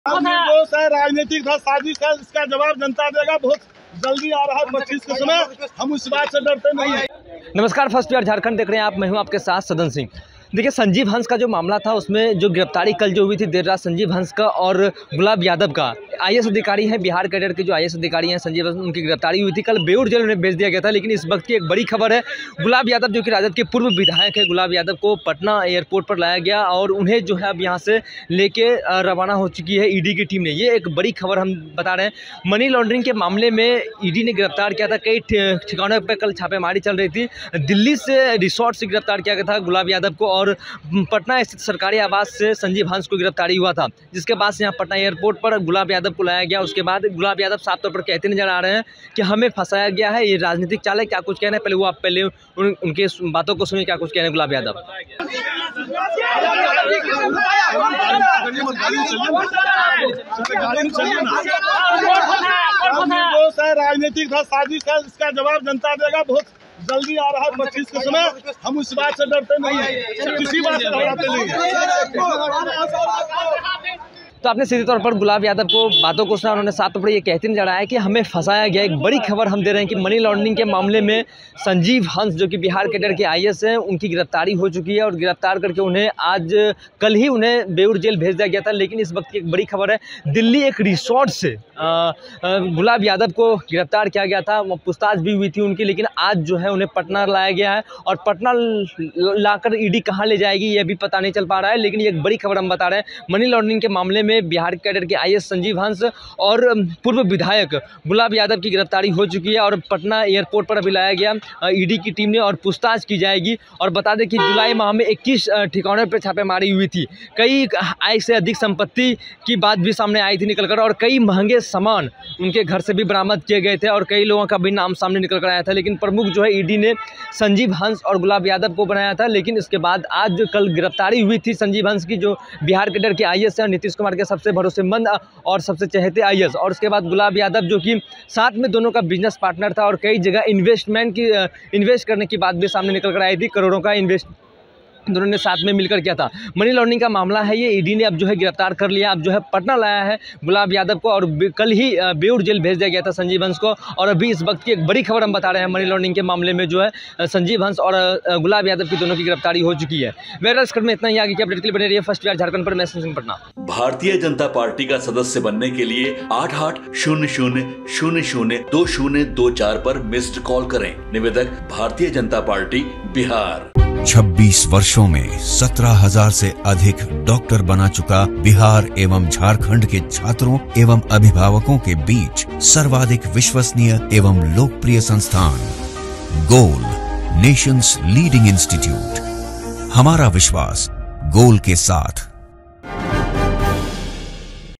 Gulab Yadav को लेकर Patna पहुंची ED की टीम अब जायंगे जेल
गुलाब यादव को लेकर पटना पहुंची ED की टीम अब जायंगे जेल
First Bihar News :

Other Trending Videos:

 1.Anant Singh का दिखा देशी अंदाज,मोकामा में सजी महफिल,Bhojpuri गाने पर पर झूमने लगे पूर्व MLA